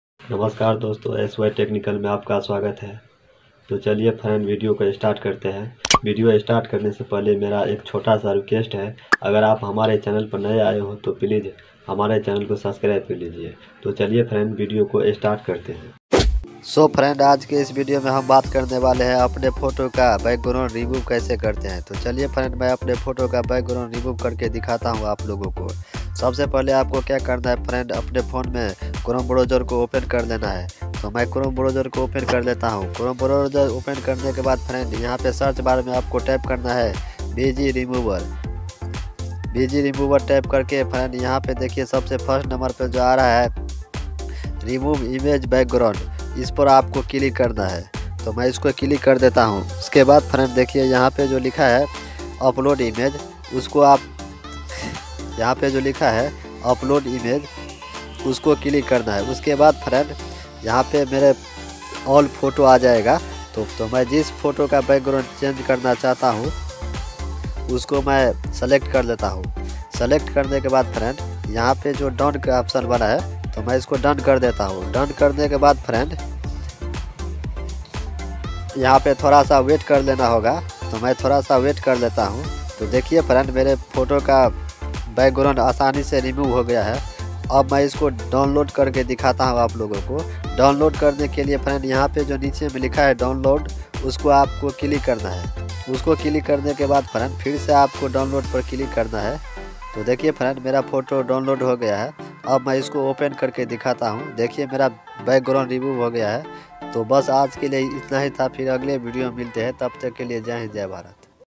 How to Remove Photo Background in One Click | HD Quality || Photo ka Background Kaise Hataye
Thank you all my friend ❤️❤️❤️